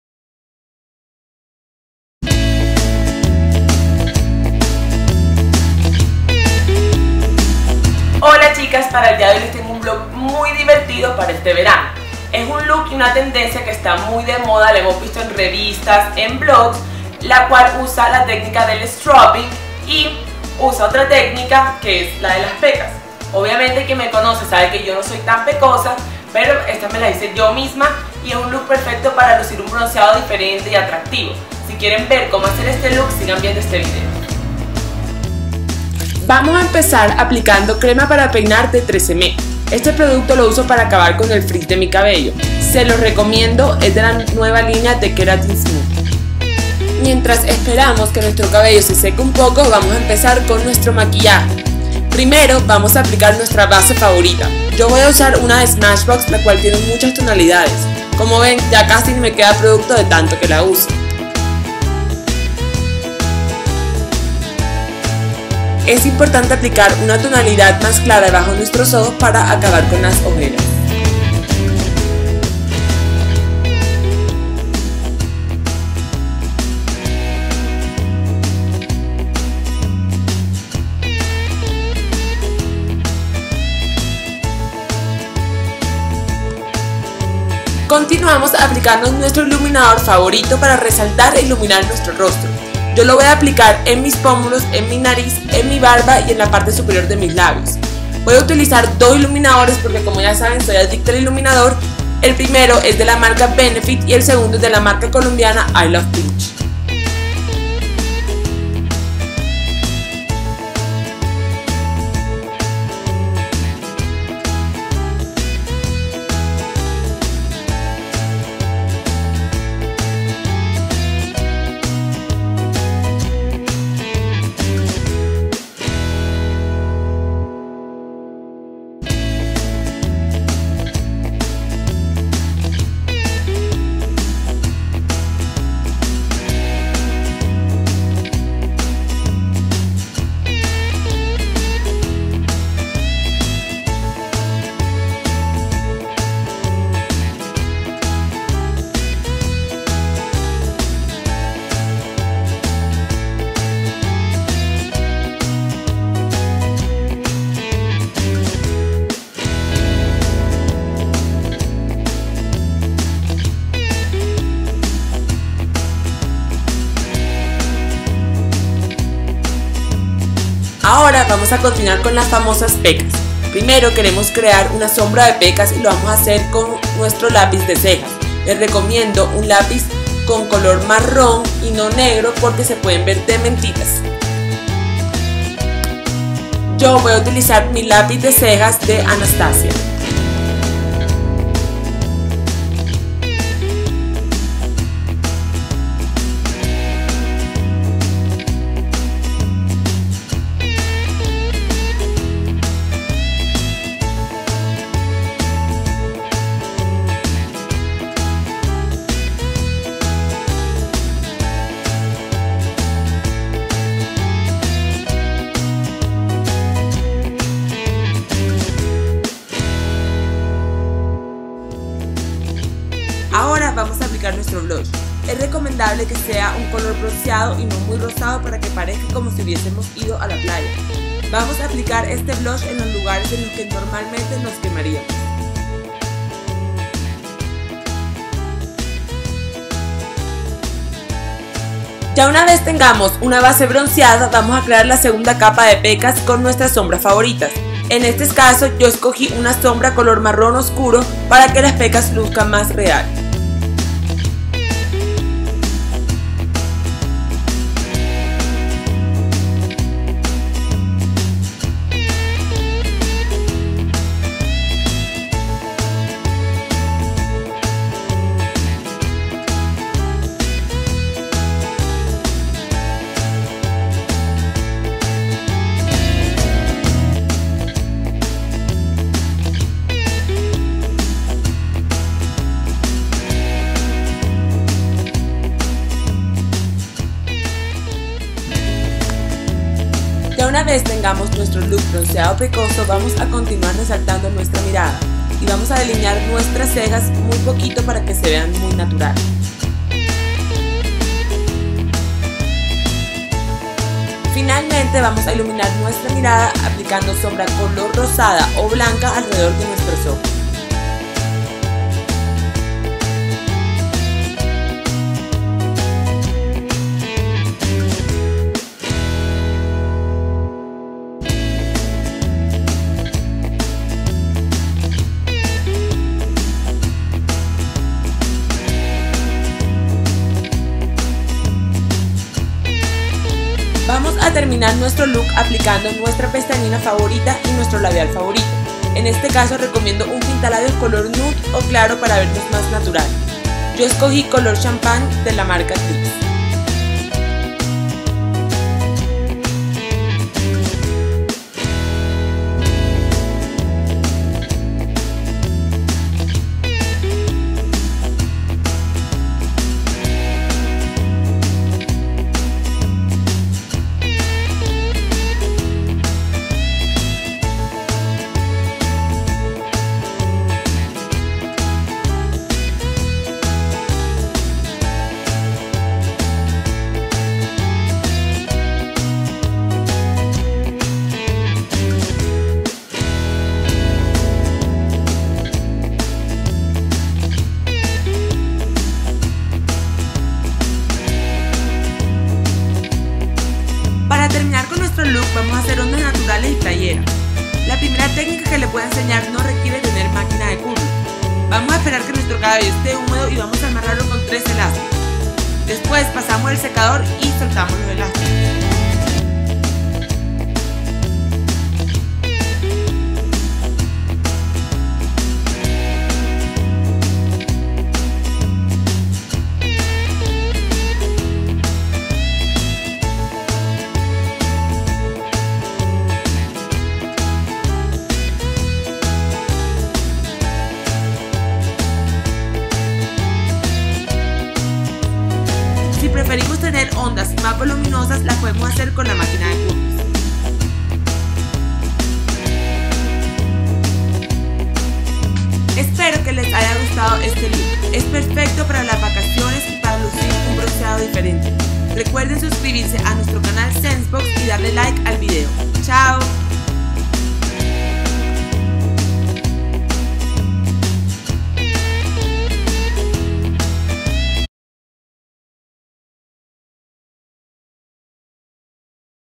Look Natural: Bronceado Pecoso
Hola a todas!
Hoy les traemos un hermoso video de nuestra creadora y bloguera Jennifer donde nos enseña a hacer un look con ondas playeras y un maquillaje natural bronceado que incluso contiene PECAS!!

Productos Usados:
Crema para Peinar Tresemmé
Base Smashbox
Iluminador Benefit
Iluminador I love Pinch
Lapiz de cejas Anastasia
Blosh Palladio
Sombras Anastasia
Rimel Benefit
Labial Glips